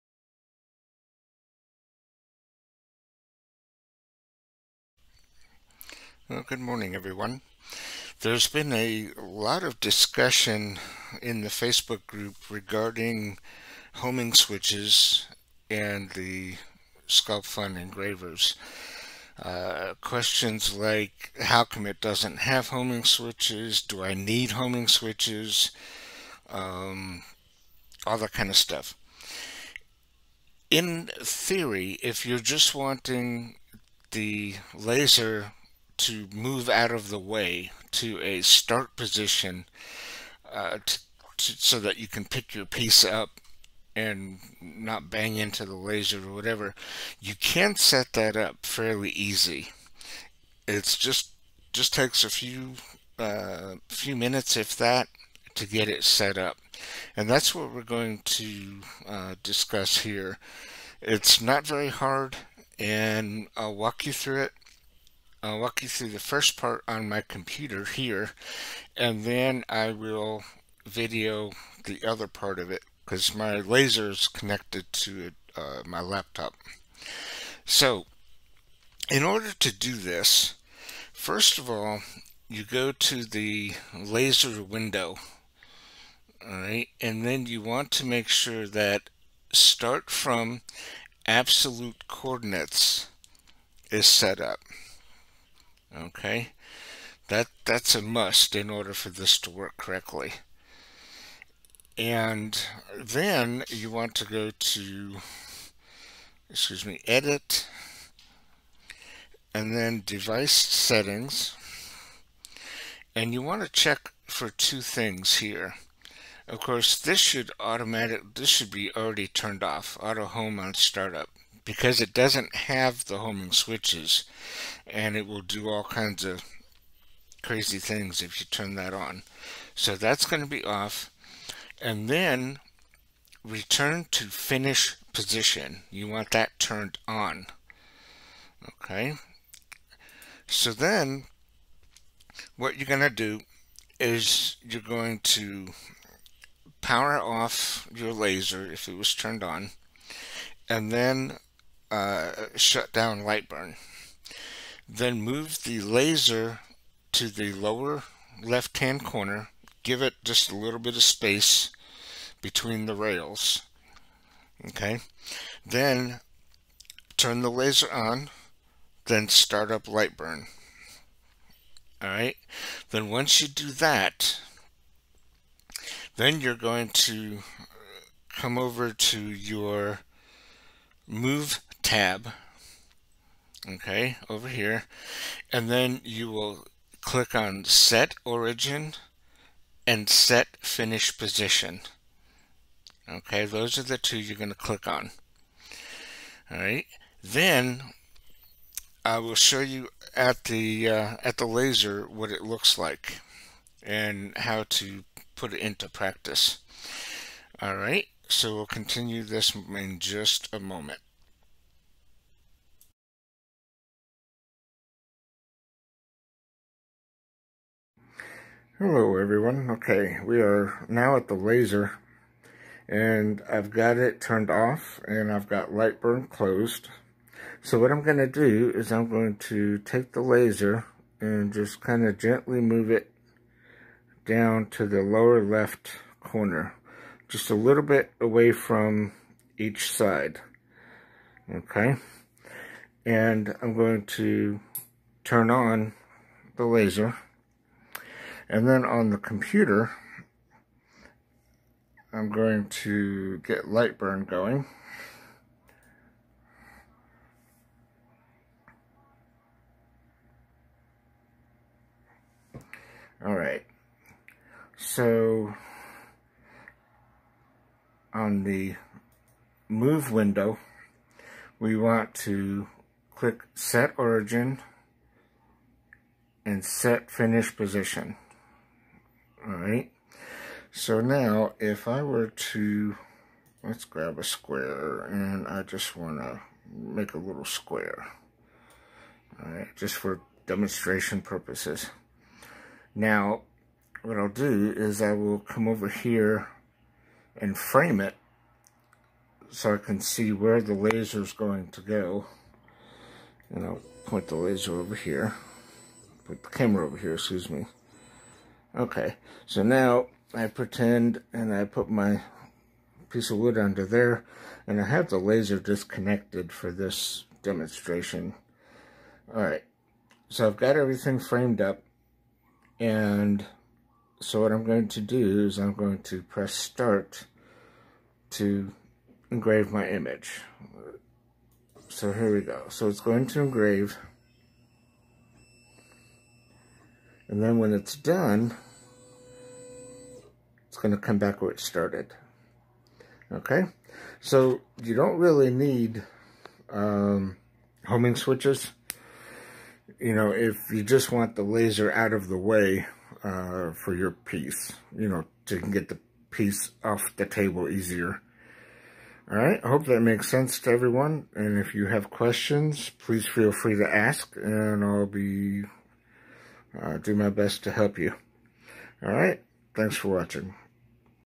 Homing Switches.. Are they needed?
A quick video regarding the topic of homing switches.. Are they needed or not?